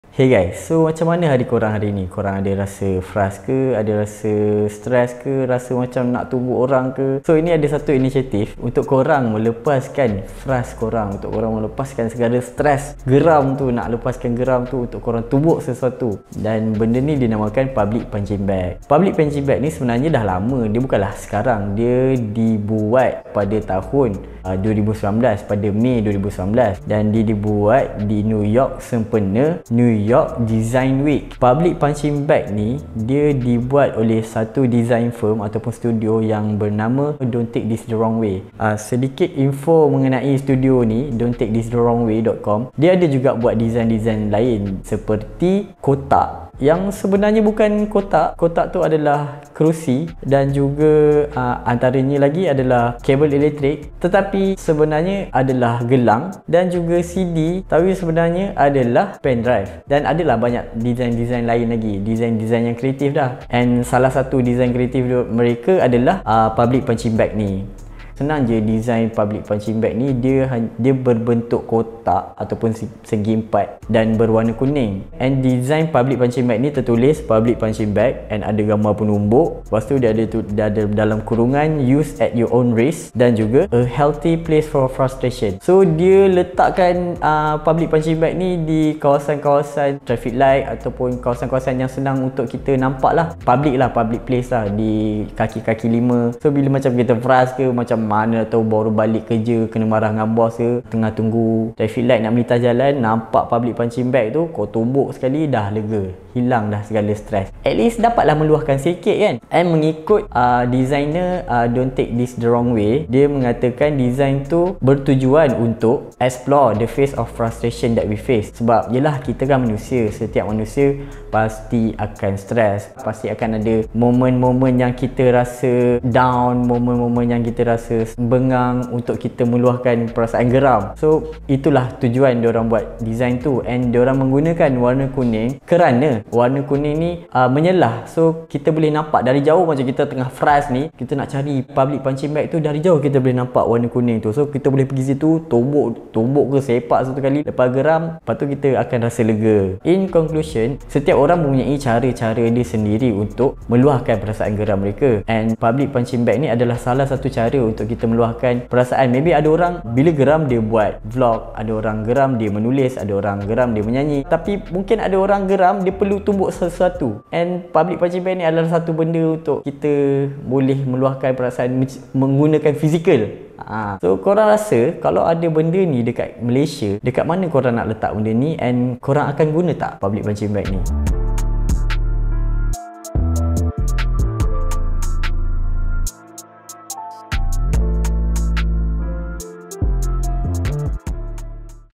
Public Punching Bag
Tau tau berlatih boxing tepi jalan.

Pengguna Baru: SHPFBB12
Pengguna sedia ada: 1212PHSFB